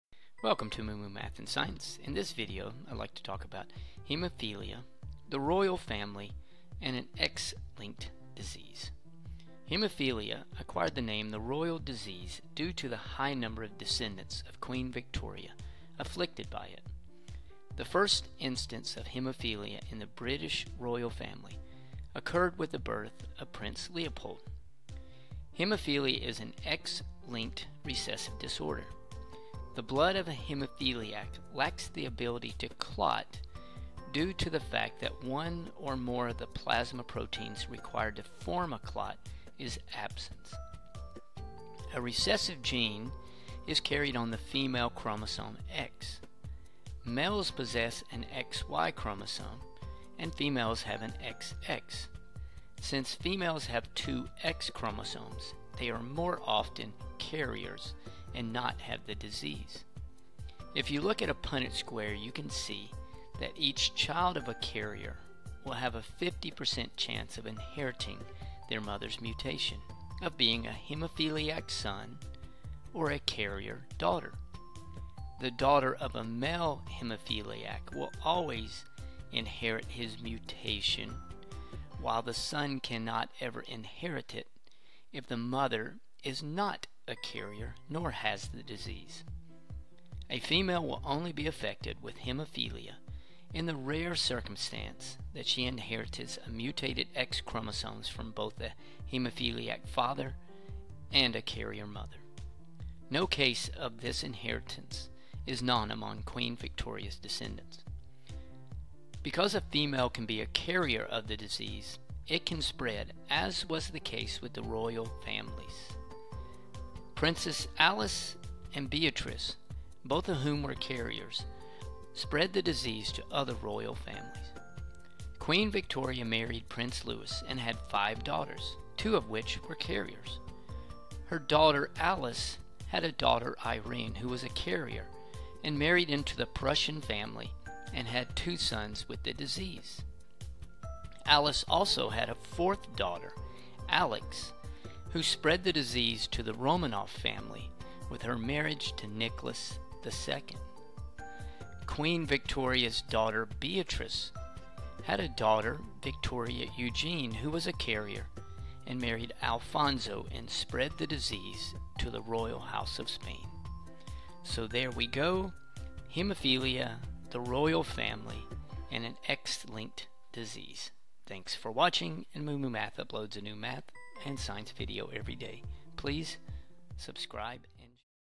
Hemophilia Royal Family ( x-linked disease example)
Hemophilia is an X-linked recessive disorder. The blood of a hemophiliac lacks the ability to clot, due to the fact that one or more of the plasma proteins required to form a clot is absent or reduced in their blood.
A recessive gene, it is carried on the female chromosome X . Males possess XY chromosomes and females XX. Since females have two X chromosomes, they are more often than not carriers.

If you look at the Punnett Square you can see the following.
Each child of a carrier will have a 50% chance of inheriting hemophilia.  The daughter of a male haemophiliac will always inherit the disease, while a son cannot ever inherit it. A female will only be affected with haemophilia in the rare circumstance that she inherits mutated X chromosomes from both a hemophiliac father and a carrier mother. No case of such double inheritance is known among Queen Victoria's descendants.
Correction:
1:47 I made a mistake. The H in the lower right box should be lower case h and not a capital letter. H